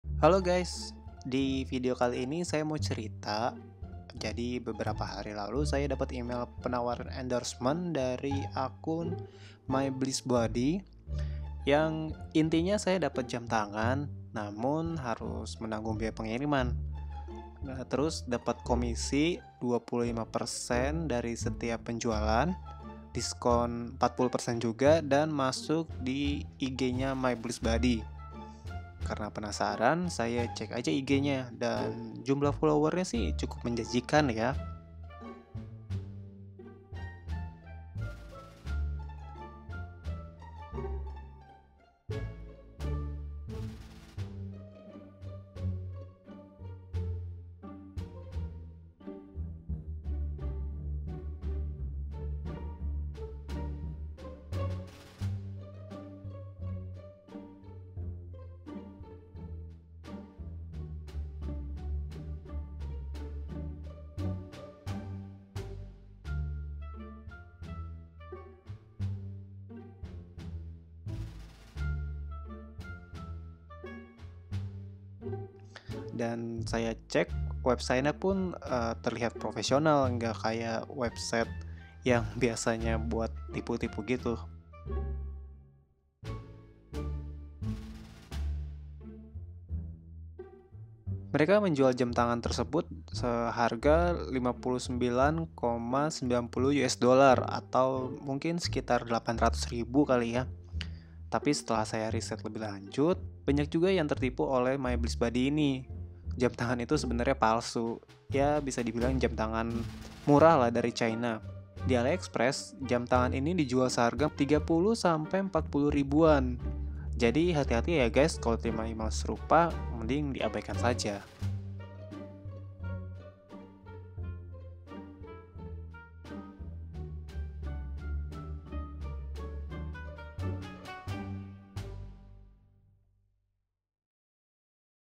Modus Penipuan Endorsement | Myblissbody SCAM
Sedikit cerita aja soal modus penipuan endorsement dari Myblissbody.